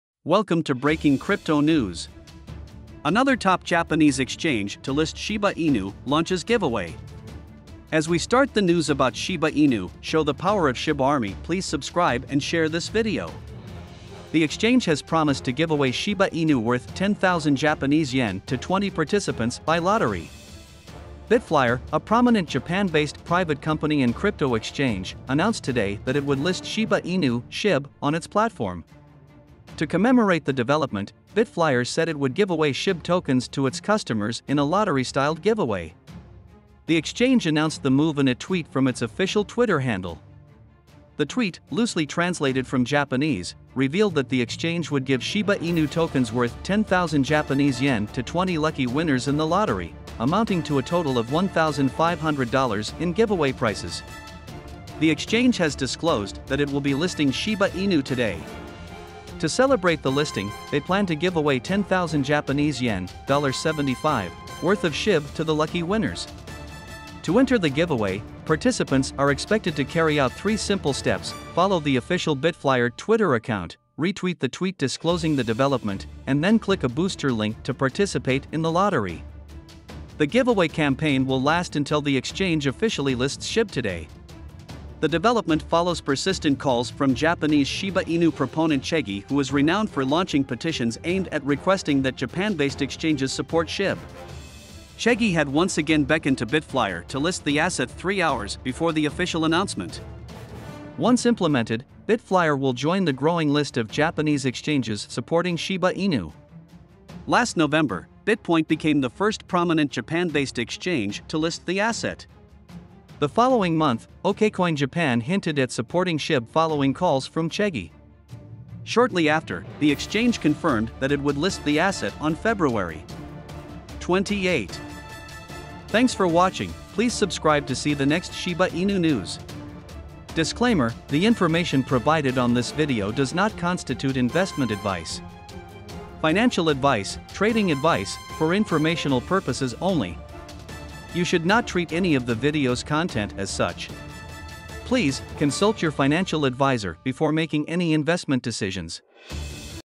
Breaking Crypto News | Another Top Japanese Exchange to List Shiba Inu, Launches Giveaway
The exchange has promised to give away Shiba Inu worth 10,000 Japanese Yen to 20 participants by lottery.

to read the original text on thecryptobasic.com
( Guides, Crypto News, Exchanges, Trading Bots, Crypto Trading Guide.)
BitFlyer, a prominent Japan-based private company and crypto exchange, announced today that it would list Shiba Inu (SHIB) on its platform. To commemorate the development, BitFlyer said it would give away SHIB tokens to its customers in a lottery-styled giveaway.
The exchange announced the move in a tweet from its official Twitter handle. The tweet, loosely translated from Japanese, revealed that the exchange would give Shiba Inu tokens worth 10,000 Japanese Yen to 20 lucky winners in the lottery, amounting to a total of $1,500 in giveaway prices.
The exchange has disclosed that it will be listing Shiba Inu today. To celebrate the listing, they plan to give away 10,000 Japanese Yen (~$75) worth of SHIB to the lucky winners.
To enter the giveaway, participants are expected to carry out three simple steps: follow the official BitFlyer Twitter account, retweet the tweet disclosing the development, and then click a Booster link to participate in the lottery. The giveaway campaign will last until the exchange officially lists SHIB today.
The development follows persistent calls from Japanese Shiba Inu proponent Cheggy who is renowned for launching petitions aimed at requesting that Japan-based exchanges support SHIB. Cheggy had once again beckoned to BitFlyer to list the asset three hours before the official announcement.
Once implemented, BitFlyer will join the growing list of Japanese exchanges supporting Shiba Inu. Last November, BitPoint became the first prominent Japan-based exchange to list the asset. The following month, OKCoin Japan hinted at supporting SHIB following calls from Cheggy. Shortly after, the exchange confirmed that it would list the asset on Feb. 28.